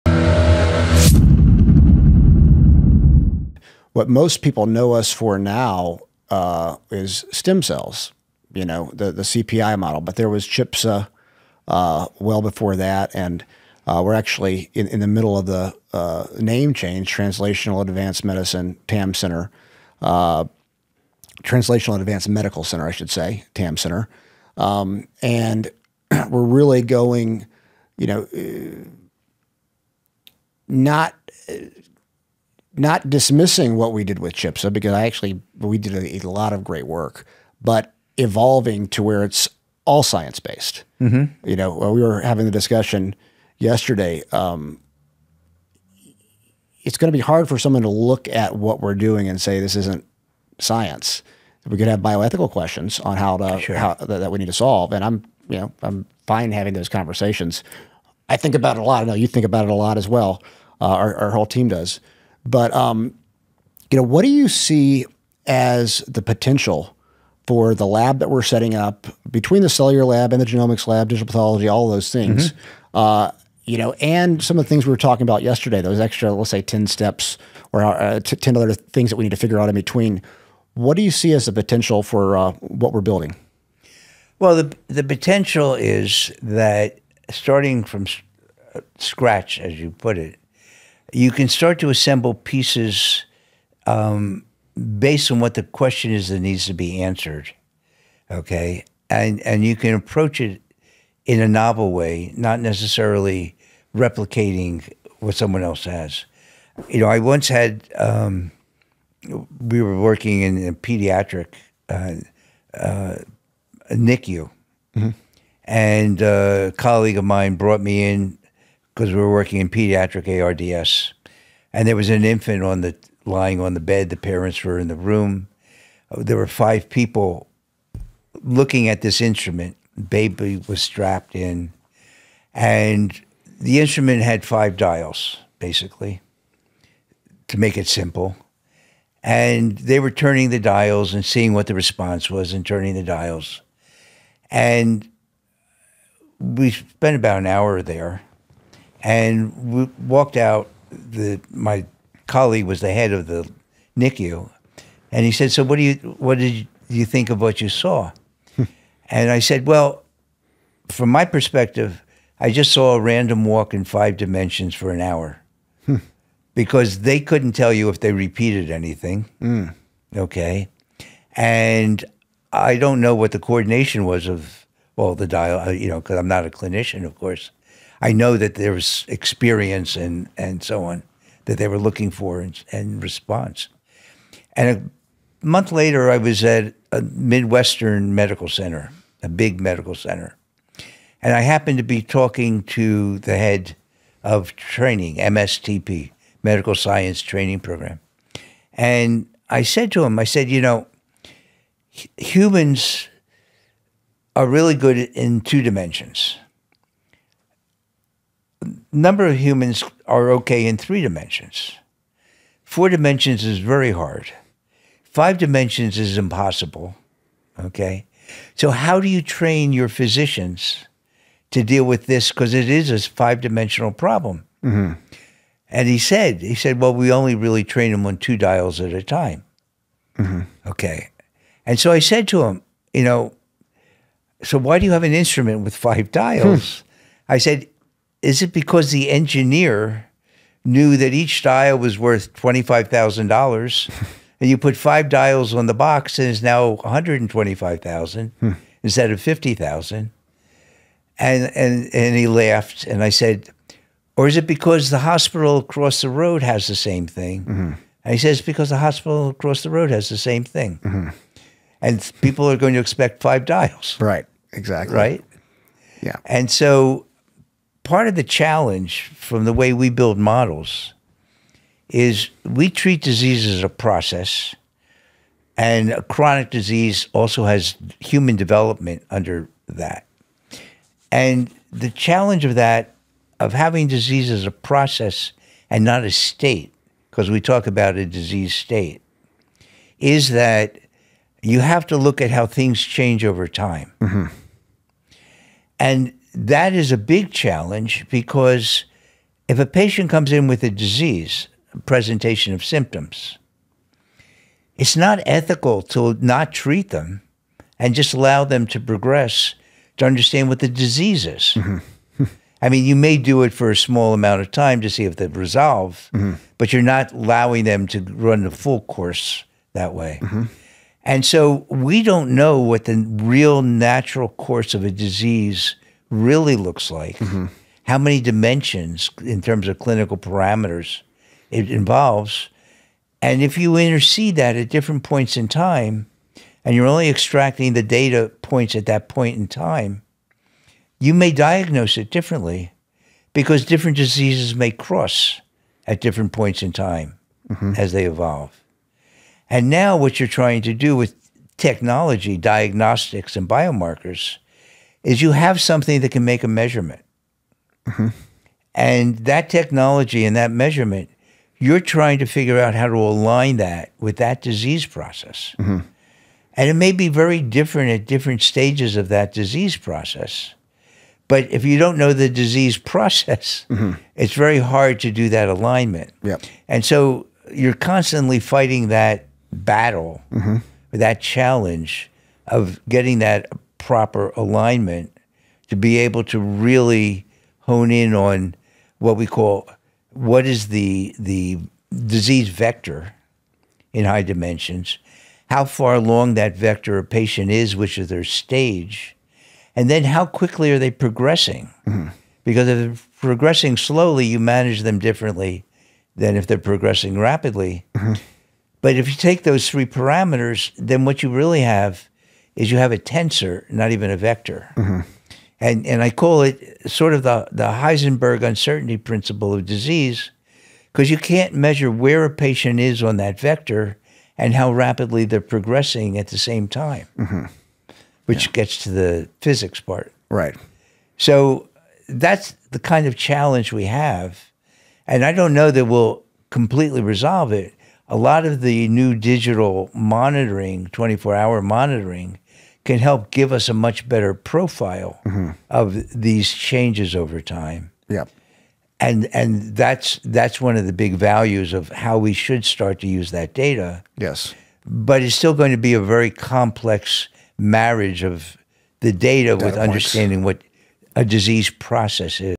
The New TAM Center in Tijuana, Mexico is Revolutionizing Medicine
Why The TAM Center in Tijuana, Mexico has the opportunity to set the standard for medical treatment by starting from scratch.